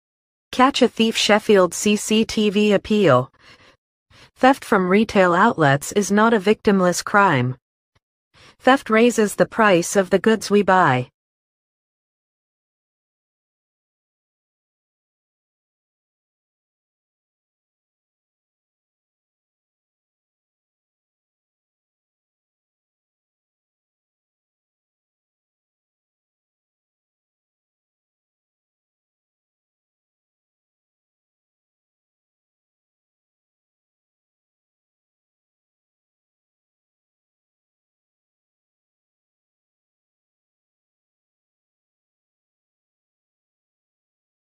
Fresh CCTV Appeal Spar Retailer Sheffield!
Male suspect wanted for questioning by the Police in connection with theft.

Could he of been in your store?

On the 9 August 2022 a lone male suspect enters our members retail premises in Hackenthorpe, Sheffield and selected a bottle of wine and 4 x crates of Carling Black Label quickly exiting the store without making payment.

After analysing the footage we noted that male suspect walked around the store very quickly trying to avoid certain cameras by adjusting his hood and lowering his head to try and avoid the CCTV detecting his face.

The suspect was wearing a green hooded Puma top, a red face mask, light brown tracksuit bottoms and grey trainers.

If you can provide any information regarding this suspect please use reference number: SH090822

Footage of the suspected incident will be published via the Catch a Thief website at a later date.